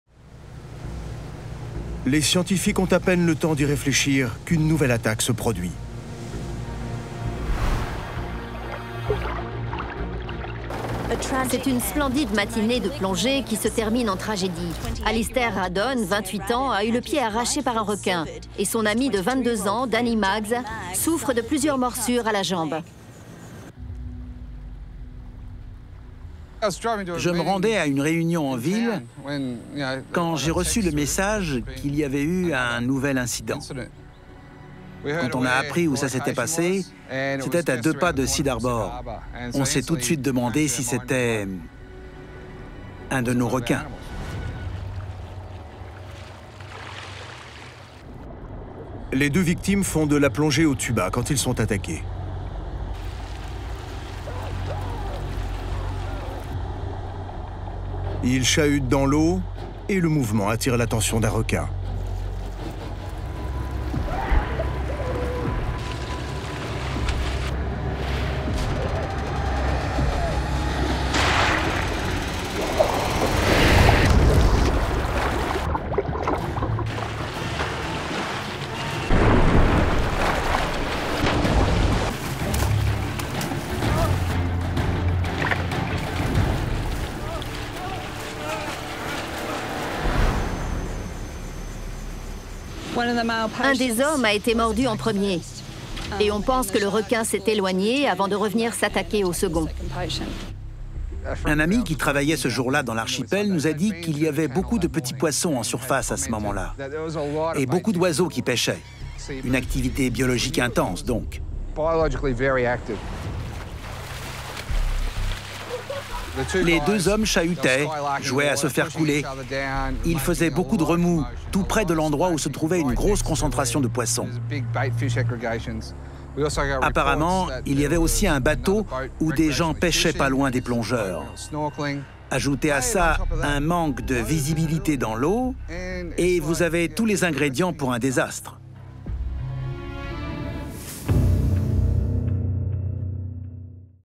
Cid Harbour : un requin attaque deux plongeurs
Près de la baie de Cid Harbour, deux hommes font de la plongée au tuba et chahutent dans l'eau. Le remous finit par attirer un requin qui les attaque à de multiples reprises.